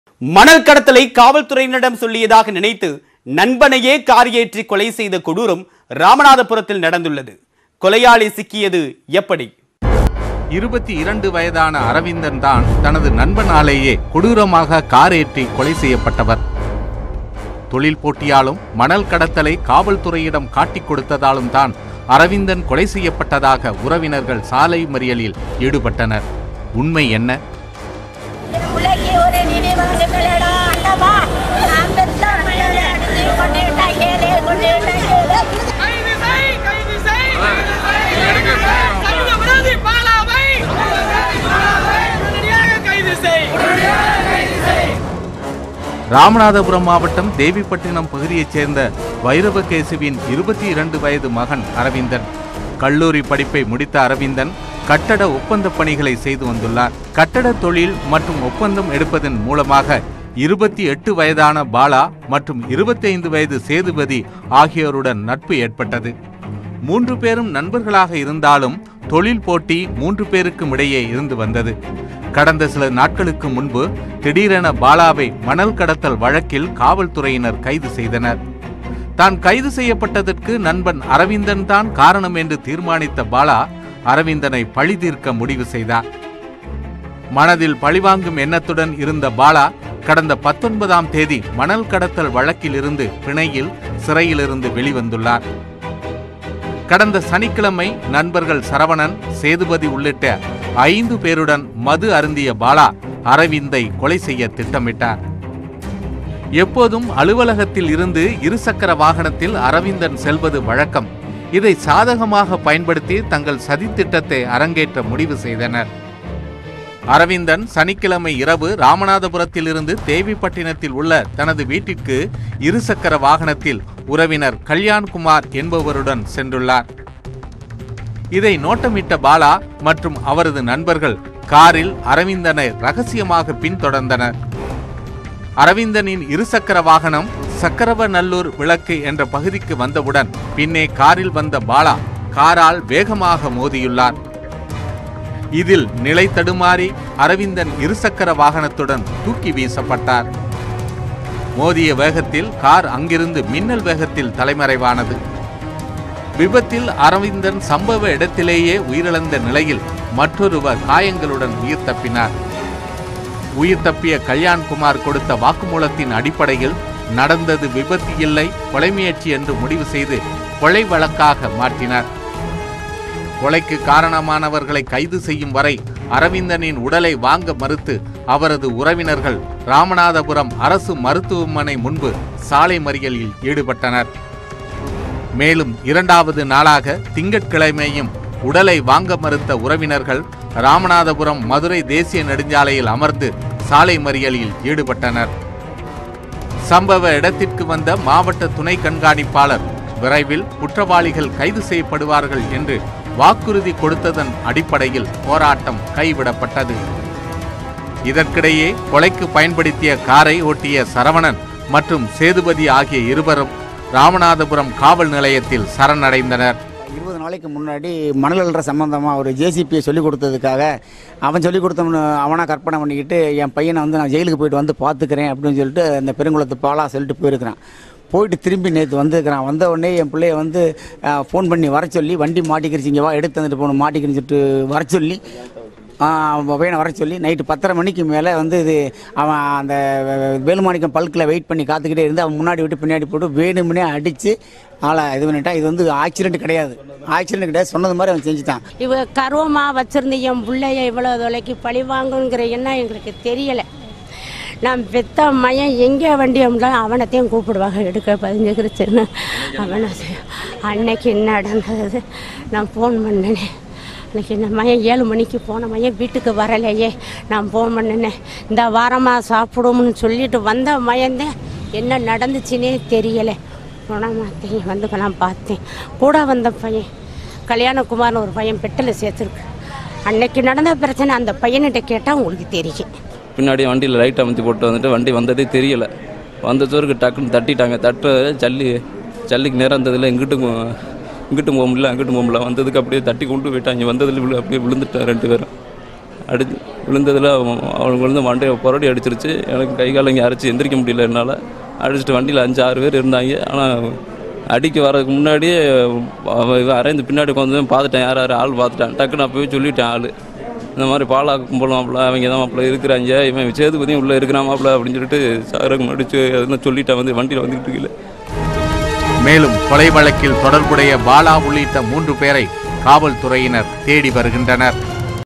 Crime Time: மணல் கடத்தல் மோதல் - இளைஞர் கார் ஏற்றிக் கொலை
மணல் கடத்தலை காவல்துறையிடம் சொல்லியதாக நண்பனையே கார் ஏற்றிக் கொலை செய்த கொடூரம் ராமநாதபுரத்தில் நடந்துள்ளது. கொலையாளி சிக்கியது எப்படி?